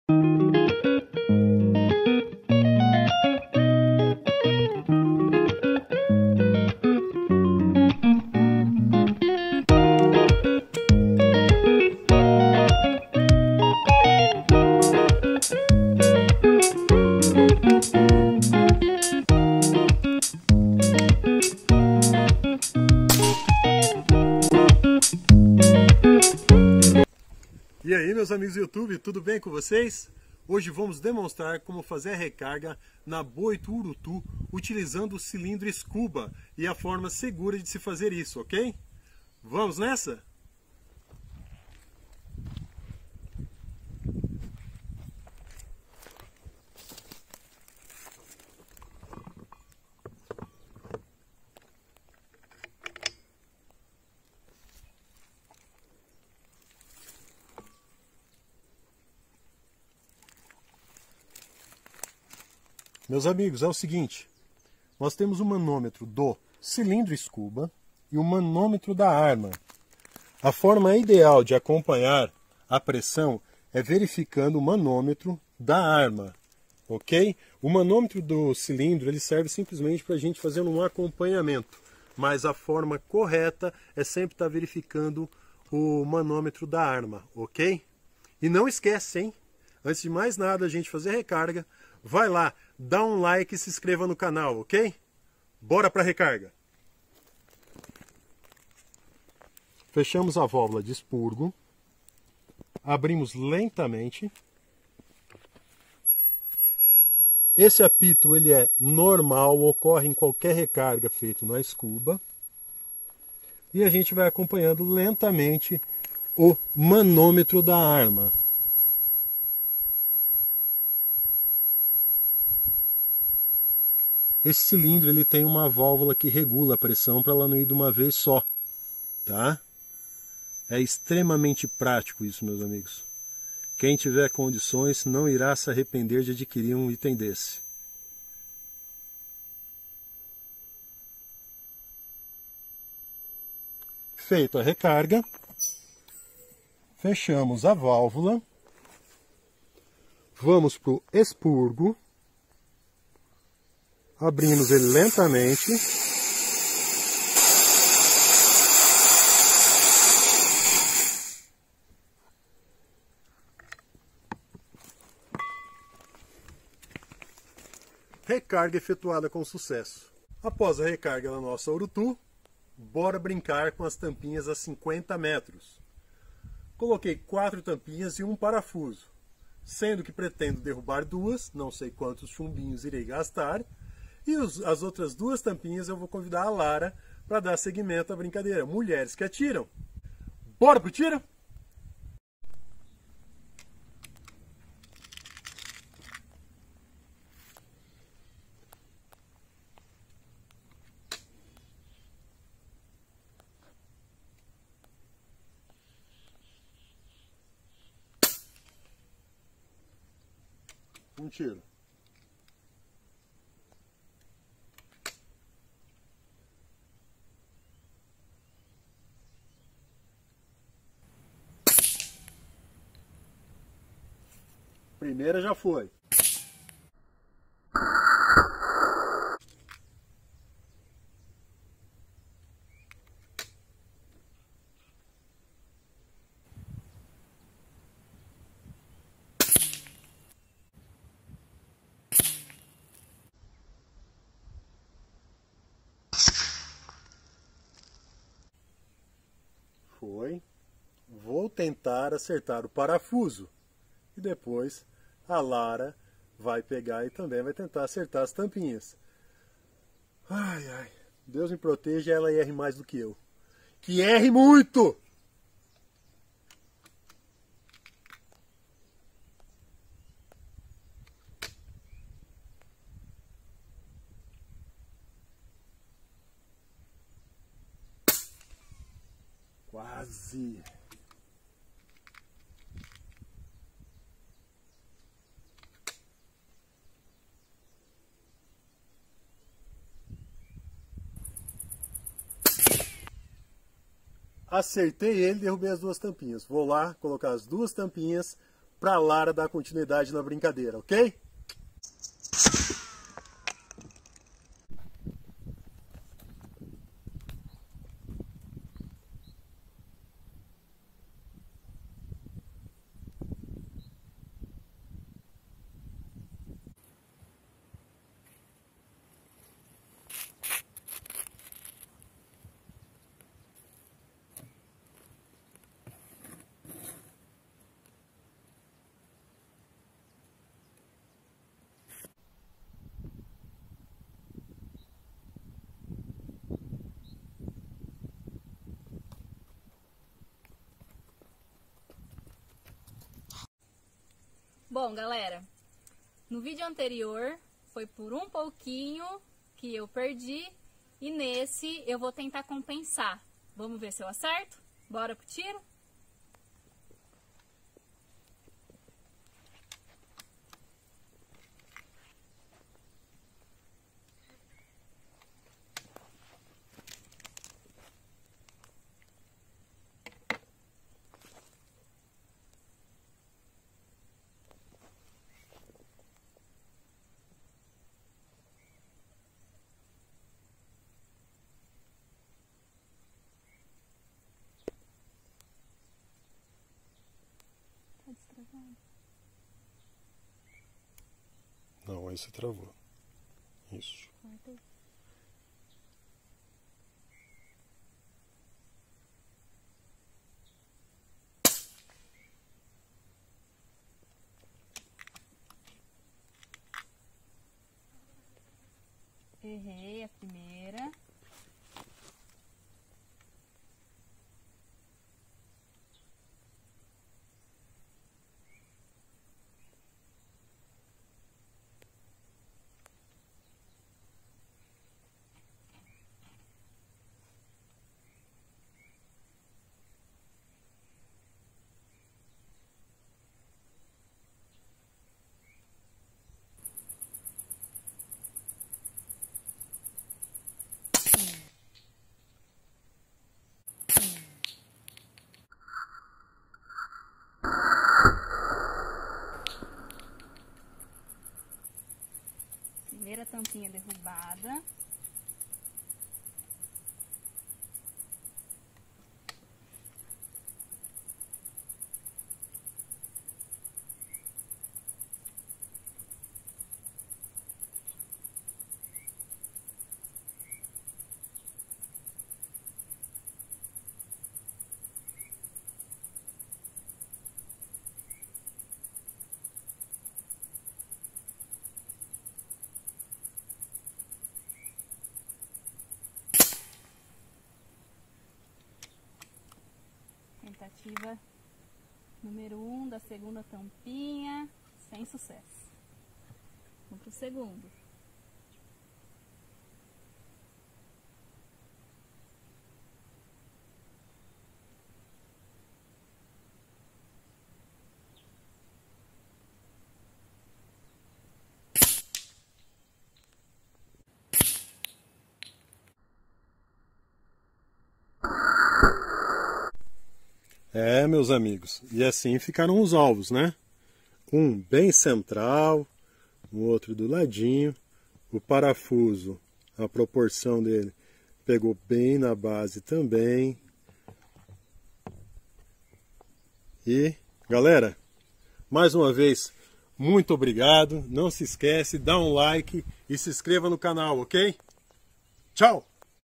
COMO FAZER A RECARGA COM CILINDRO SCUBA NA BOITO URUTU 931 e logo depois tiros a 50m
Olá meus amigos hoje vamos demonstrar como fazer a recarga com  cilindro scuba  na PCP  Boito Urutu 931 nossa rainha da precisão e faremos disparos a 50 metros usando os chumbos Rifle Round e  com a participação especial da nossa atiradora Lara  no vídeo pois ela manda muito bem no tiro .

boa diversão a todos

Bora pro tiro